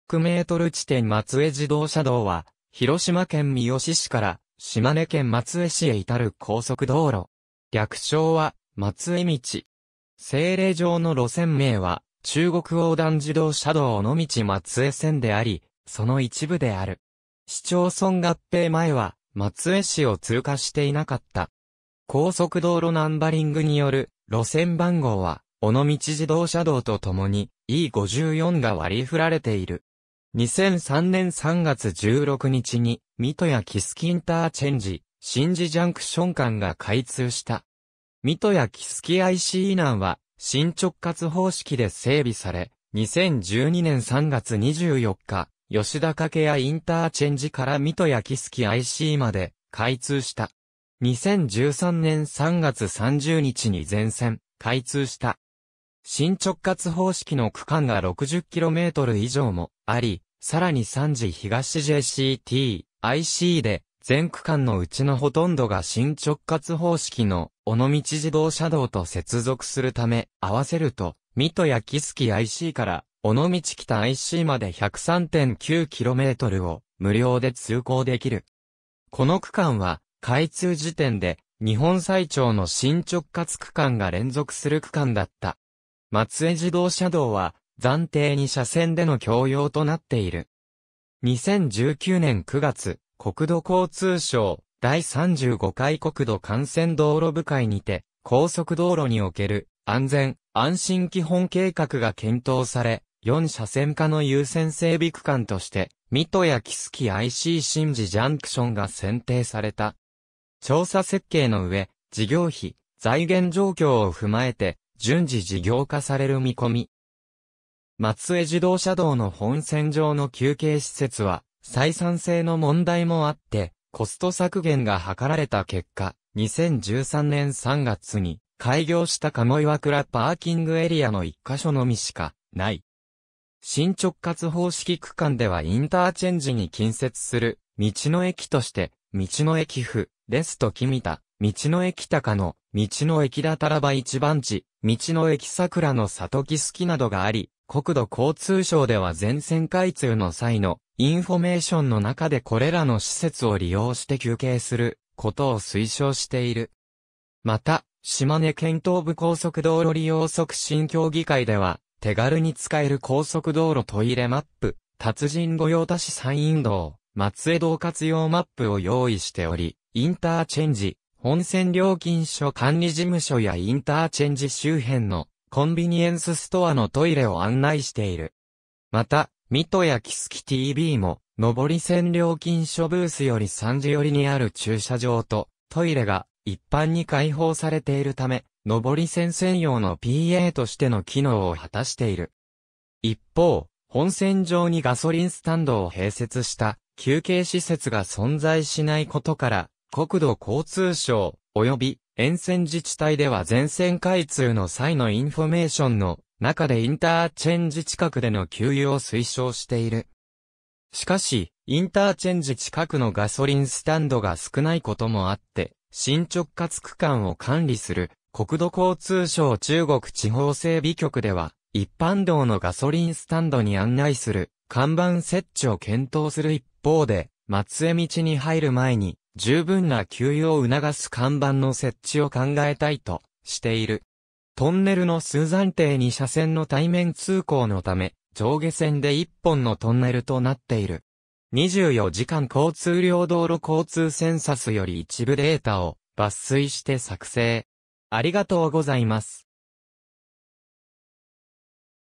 松江自動車道
口和インターチェンジから南約700メートル地点（広島県庄原市、松江方面） 松江自動車道（まつえじどうしゃどう、MATSUE EXPRESSWAY）は、広島県三次市から島根県松江市へ至る高速道路（高速自動車国道）。
略称は松江道（まつえどう、MATSUE EXPWY）。
政令上の路線名は「中国横断自動車道尾道松江線」（広島県尾道市 - 島根県松江市）であり、その一部である。
市町村合併前は松江市を通過していなかった。
高速道路ナンバリングによる路線番号は、尾道自動車道とともに「E54」が割り振られている。
2003年（平成15年）3月16日に三刀屋木次インターチェンジ (IC) - 宍道ジャンクション (JCT) 間が開通した。
三刀屋木次IC以南は新直轄方式で整備され、2012年（平成24年）3月24日、吉田掛合ICから三刀屋木次ICまで開通した。
2013年（平成25年）3月30日に全線開通した。
新直轄方式の区間が60キロメートル (km) 以上もあり、さらに三次東JCT/ICで全区間のうちのほとんどが新直轄方式（無料区間）の尾道自動車道と接続するため、合わせると三刀屋木次ICから尾道北ICまで103.9 kmを無料で通行できる。
この区間は開通時点で日本最長の新直轄区間が連続する区間だった。
松江自動車道は暫定2車線での供用となっている。
2019年（令和元年）9月、国土交通省 第35回国土幹線道路部会にて「高速道路における安全・安心基本計画」が検討され、4車線化の優先整備区間として、三刀屋木次IC - 宍道JCT が選定された。
調査設計の上、事業費、財源状況を踏まえて順次事業化される見込み。
松江自動車道の本線上の休憩施設は、採算性の問題もあってコスト削減が図られた結果、2013年（平成25年）3月に開業した加茂岩倉パーキングエリア（トイレ・情報提供コーナーのみ）の1か所のみしかない。
新直轄方式区間ではインターチェンジ (IC) に近接する道の駅として道の駅ふぉレスト君田（口和ICから約3.3 km）、道の駅たかの（高野IC連絡路上、2013年〈平成25年〉4月12日開設）、道の駅たたらば壱番地（雲南吉田IC連絡路上、2013年〈平成25年〉3月30日開設）、道の駅さくらの里きすき（三刀屋木次ICから約3.8 km）などがあり、国土交通省では全線開通の際のインフォメーションの中でこれらの施設を利用して休憩することを推奨している。
また、島根県東部高速道路利用促進協議会では「手軽に使える高速道路トイレマップ」「達人御用達山陰道・松江道活用マップ」を用意しており、インターチェンジ/本線料金所管理事務所やインターチェンジ周辺のコンビニエンスストアのトイレ（いずれも道路外の施設）を案内している。
また、三刀屋木次TBも、上り線料金所ブースより三次寄りにある駐車場とトイレが一般に解放されているため、上り線専用のPAとしての機能を果たしている。
一方、本線上にガソリンスタンドを併設した休憩施設が存在しないことから、国土交通省、および沿線自治体では全線開通の際のインフォメーションの中でインターチェンジ近くでの給油を推奨している。
しかし、インターチェンジ近くのガソリンスタンドが少ないこともあって、新直轄区間を管理する国土交通省中国地方整備局では一般道のガソリンスタンドに案内する看板設置を検討する一方で、松江道に入る前に十分な給油を促す看板の設置を考えたいとしている。
トンネルの数 暫定2車線の対面通行のため、上下線で1本のトンネルとなっている。
24時間交通量（台） 道路交通センサス （出典：「平成22年度道路交通センサス」・「平成27年度全国道路・街路交通情勢調査」（国土交通省ホームページ）より一部データを抜粋して作成）